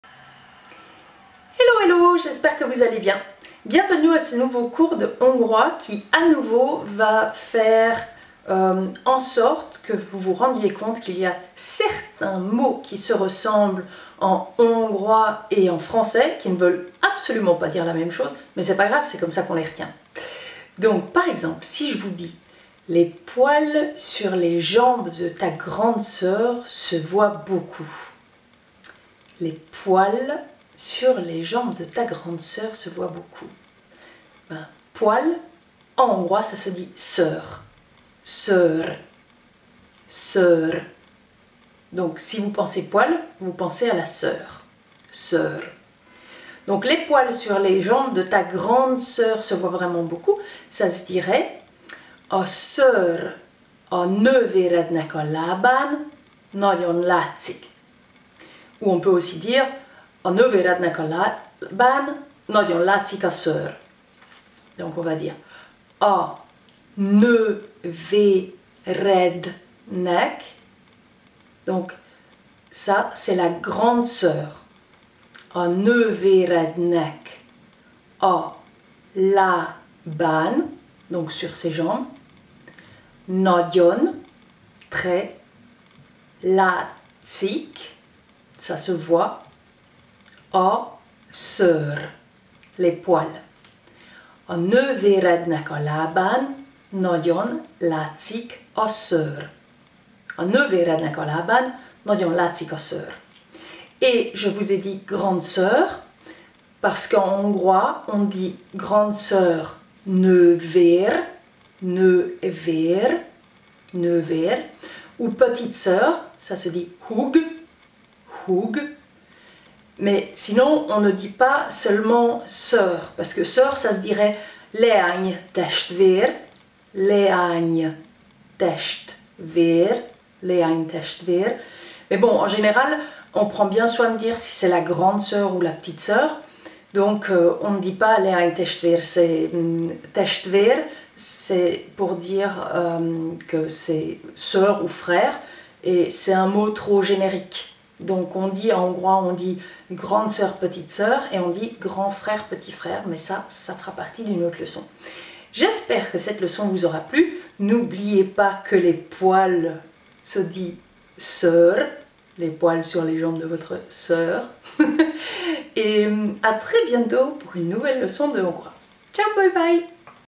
Nouveau mot en Hongrois : Poils
Si vous souhaitez me soutenir, c'est par merci!!
Un nouveau mot et une phrase d'exemple:
Szőr - poils
Léàny testvér - sœur
Nővér - grande sœur
Húg - petite sœur
Les poils sur les jambes de ta grande sœur se voient beaucoup! - A nővérednek a làbàn nagyon látcik a szőr !